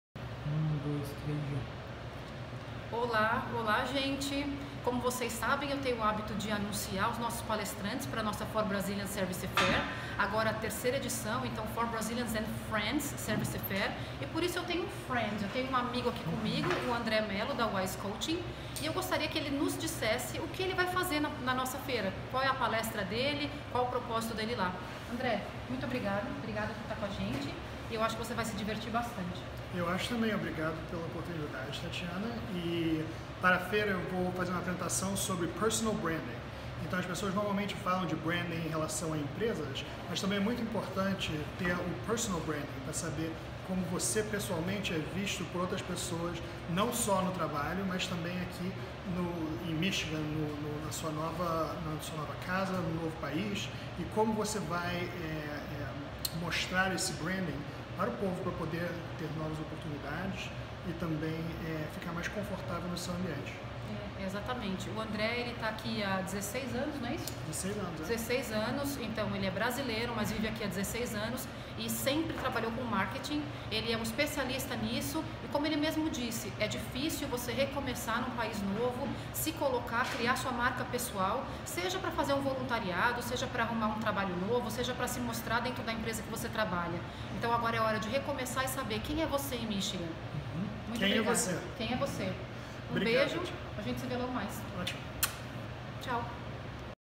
For Brazilians and Friends Service Fair - Terceira edicao - Apresentando Andre Mello
A For Brazilians Service fair e um evento gratuito criado para servir a comunidade Brasileira em Michigan